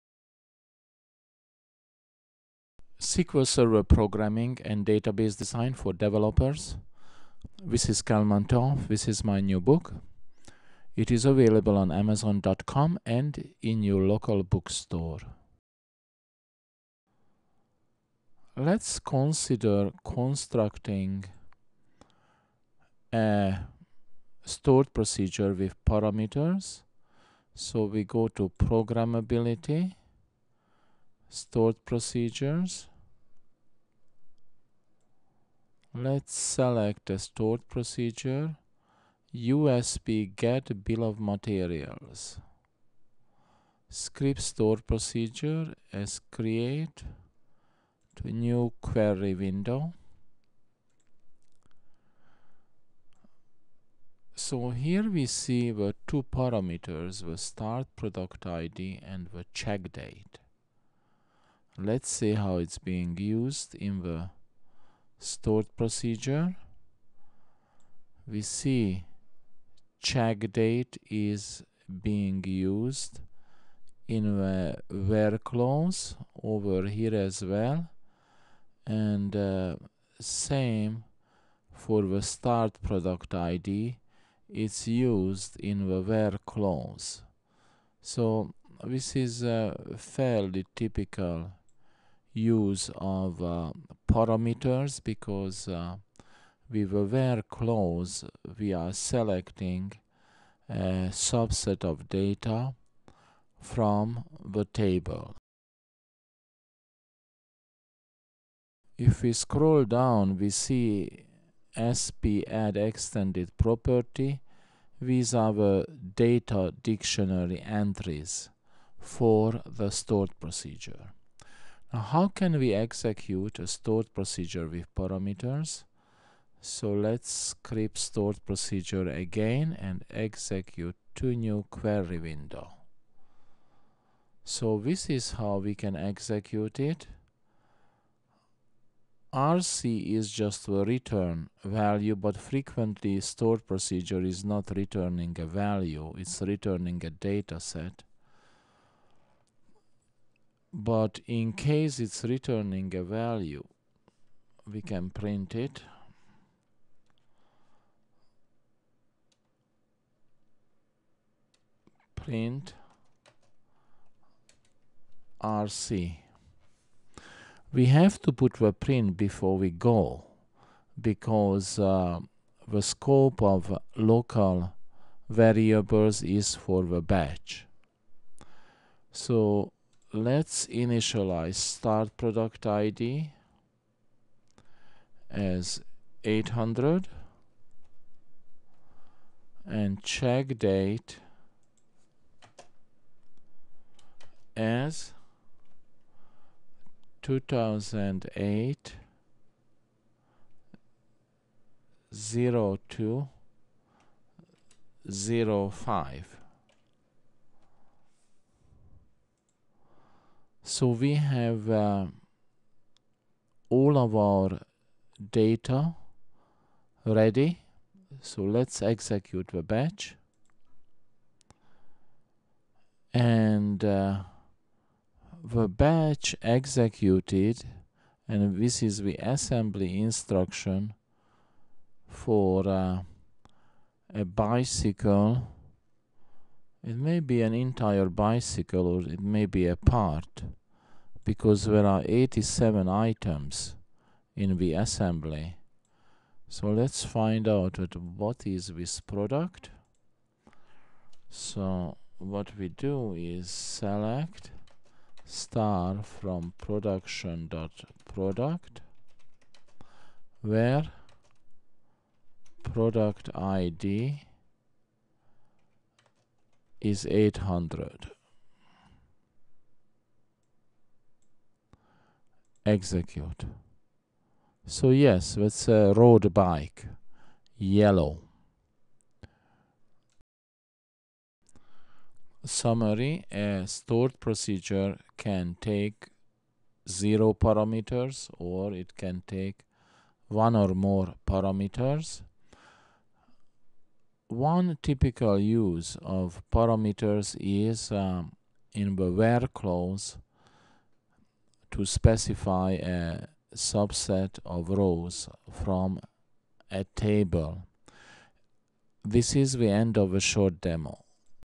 Stored Procedure with Parameters - SQL Server 2012 - Kalman Toth
Parameters use in stored procedures tutorial. Book/Kindle: SQL Programming & Database Design Using Microsoft SQL Server 2012 [Paperback] Kalman Toth (Author)